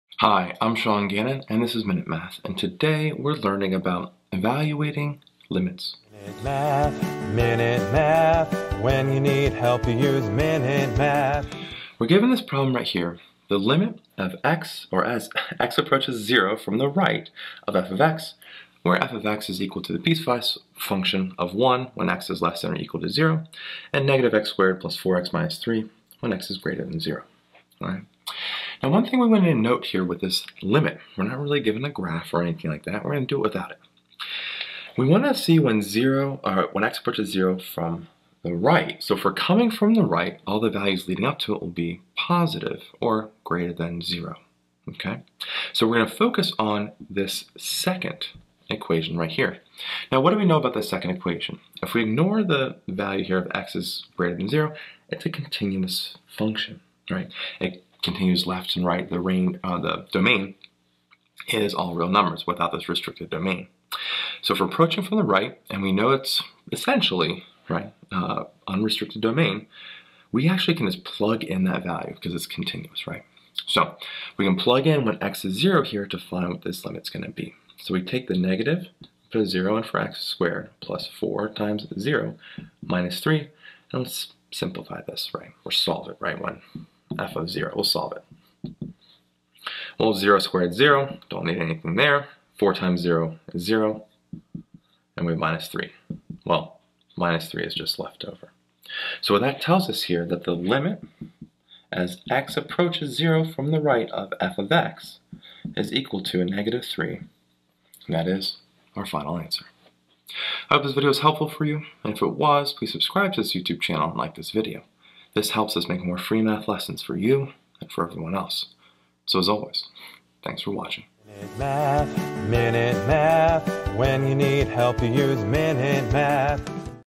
Evaluate lim x-›0^+ f(x) where f(x)={1 when x≤0; -x^2+4x-3 when x›0
Evaluating Limits. We learn how to evaluate the lim x-›0^+ f(x) where f(x)={1 when x≤0; -x^2+4x-3 when x›0. We learn how to evaluate a limit at a given point of . This is a great introduction into Limits and Calculus. This is under the topic of Limits with this free online math video lesson.